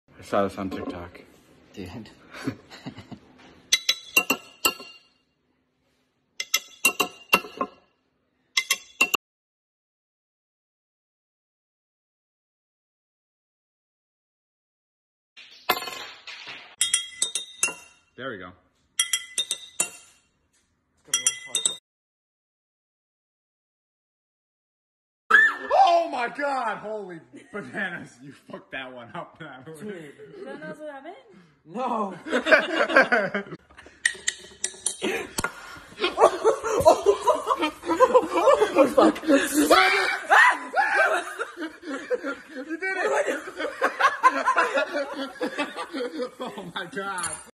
David Dobrik & Natalie open beer bottle Using Tik Tok Hack!!😲
David Dobrik